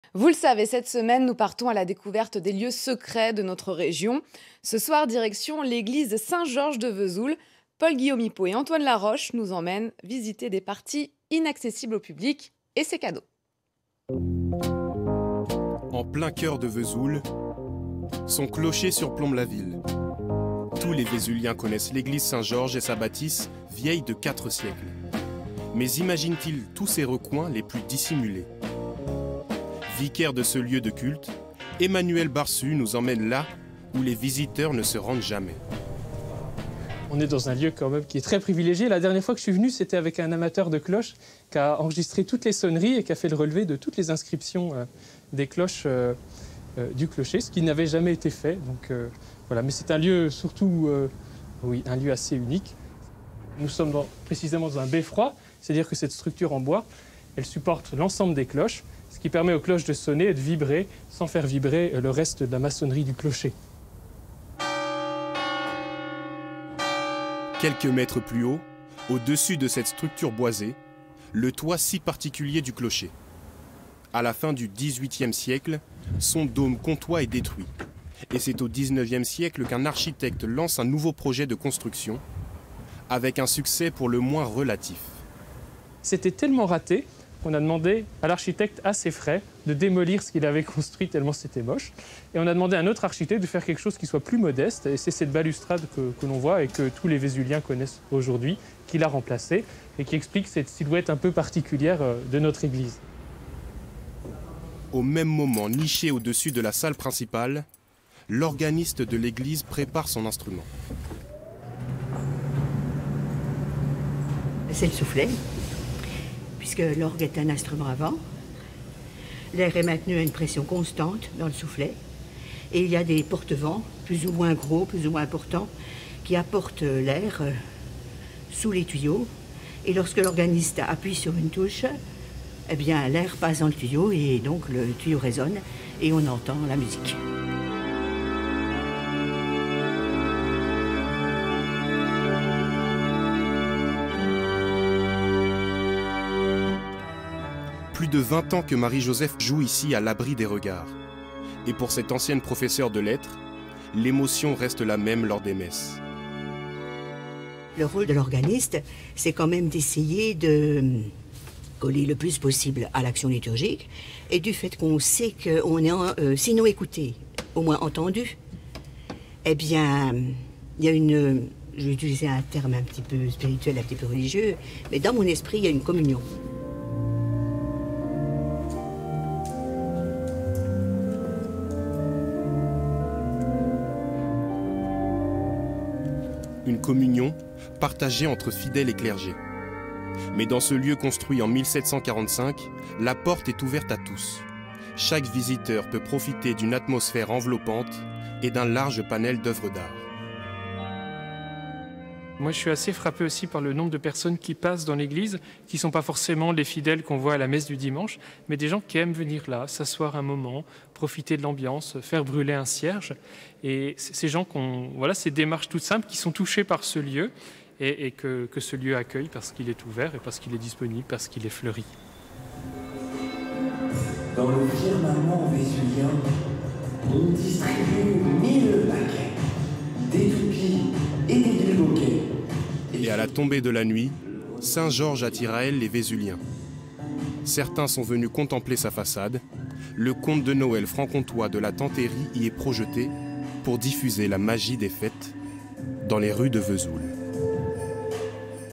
Vesoul : les petits secrets de l'église Saint-Georges
En plein cœur de Vesoul, son clocher surplombe la ville.
Tous les Vésuliens connaissent l'Église Saint-Georges et sa bâtisse, vieille de quatre siècles. Mais imaginent-ils ses recoins les plus dissimulés.
Vicaire de ce lieu de culte, Emmanuel Barsu nous emmène là où les visiteurs ne se rendent jamais, notamment le clocher, et les entrailles de l'orgue.

Intervenants :
Emmanuel Barsu, vicaire de l'Eglise Saint-Georges
Marie-Josèphe Petithuguenin, organiste